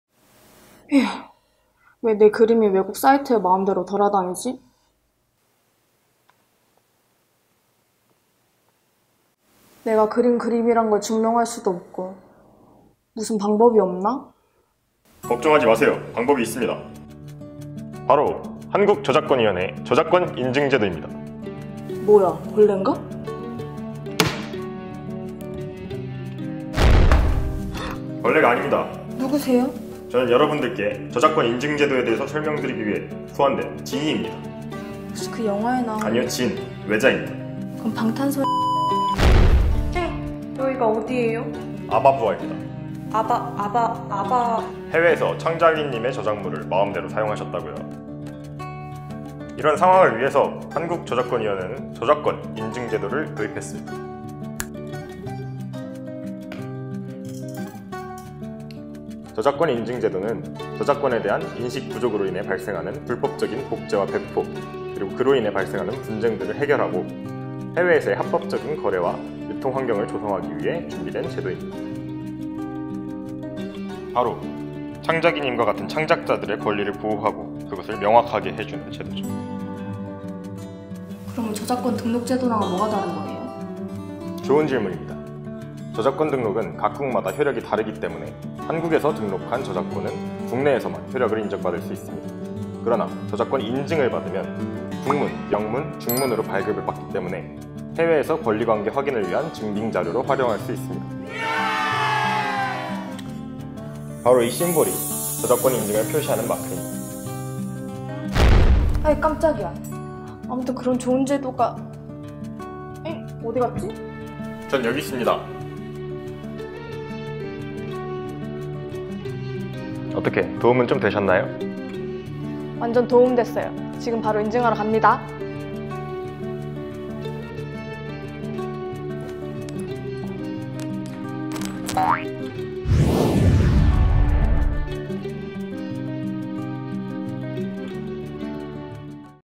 홀연히 나타난 당신은...진...?/대학생저작권서포터즈/특이성
외국사이트에 자신이 그린 그림이 마음대로 업로드 되어 속상한 창작이 앞에
홀연히 나타난 알 것 같지만 알 수 없는 정체의 그, 저작권인증제도.. 뭐라고요?

2019 대학생저작권서포터즈 8월 두 번째, 특이성(1팀)